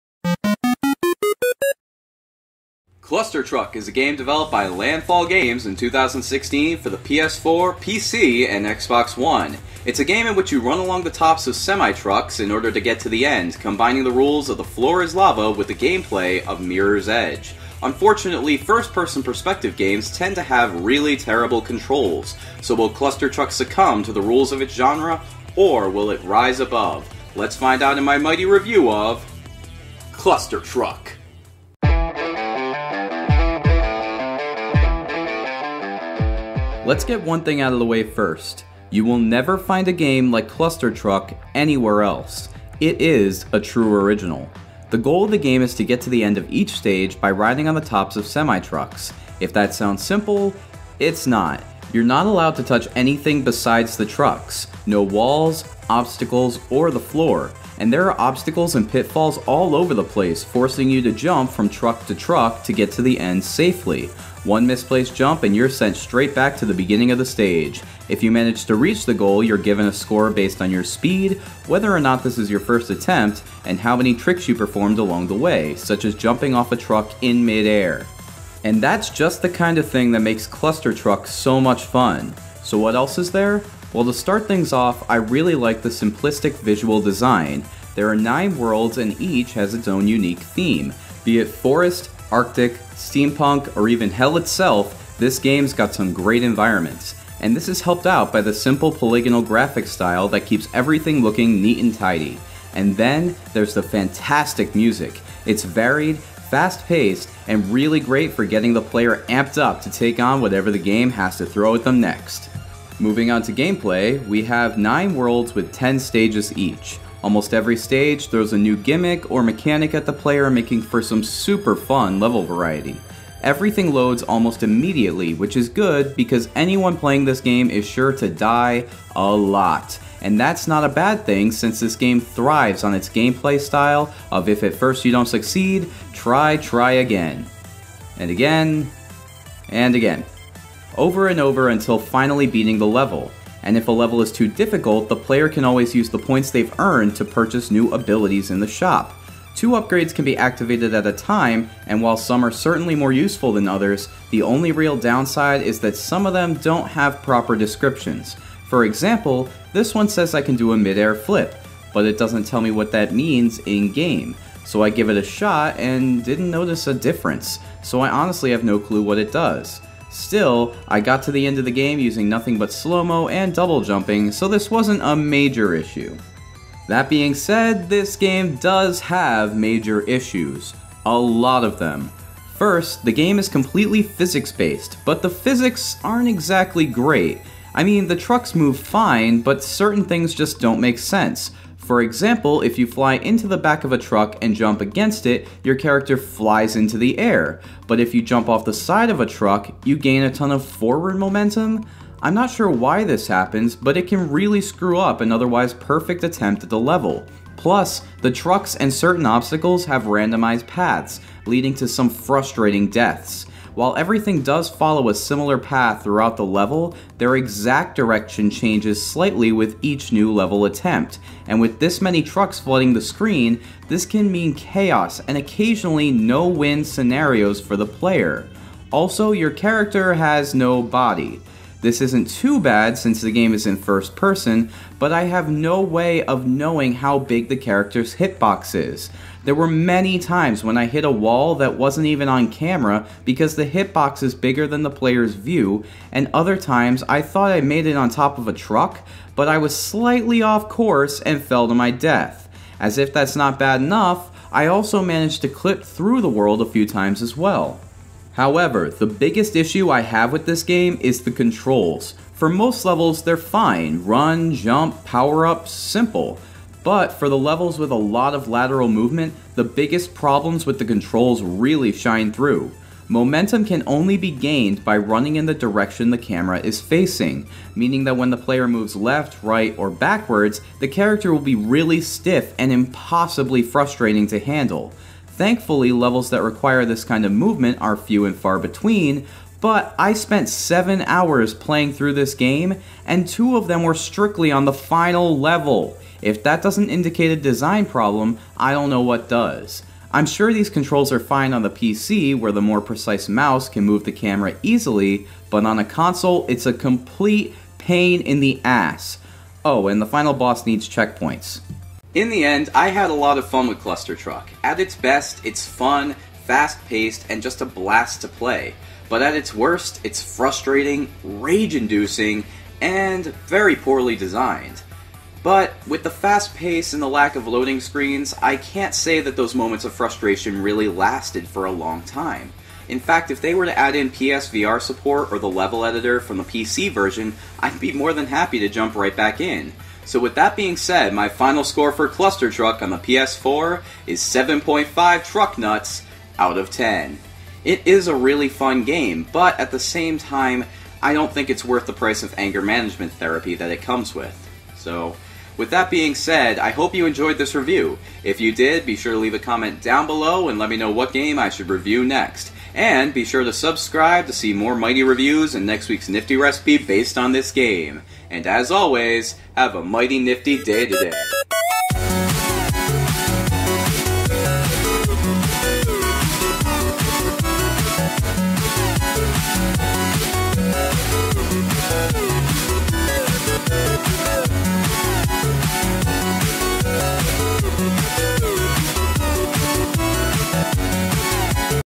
Clustertruck Review | MightyNifty
My Mighty Review of the incredibly insane and unbelievably frustrating game, Clustertruck!